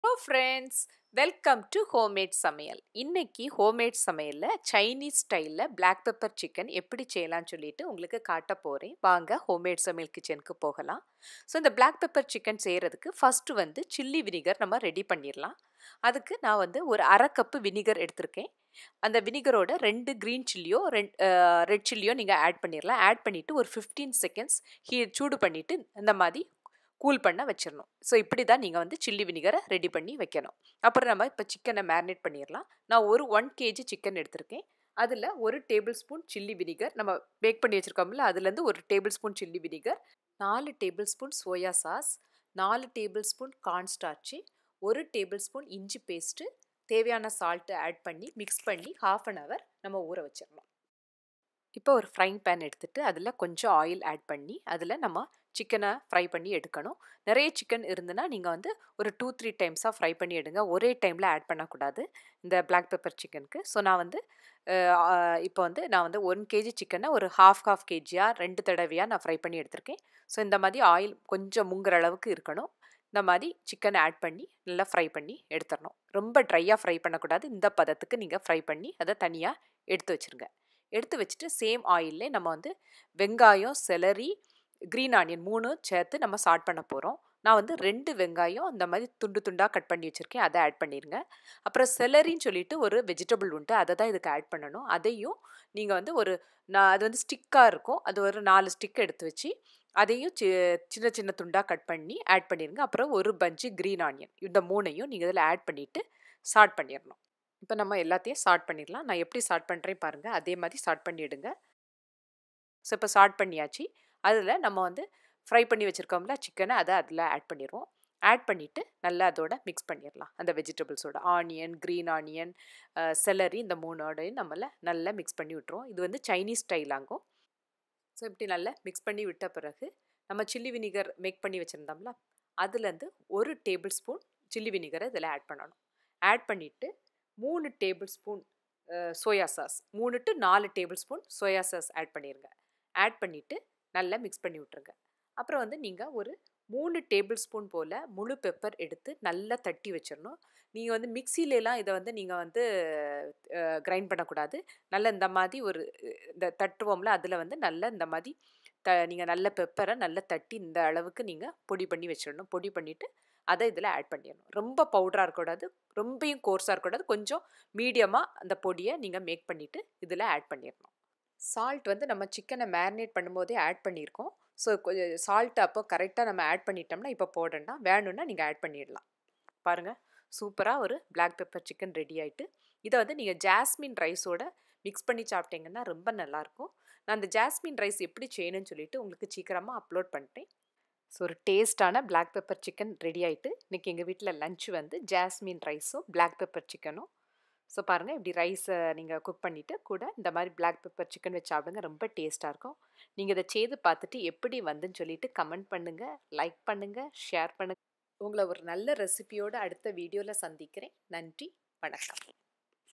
Chinese Style Black Pepper Chicken Recipe || In Tamil || Homemade Samayal
Tasty Black Pepper chicken with Jasmine Rice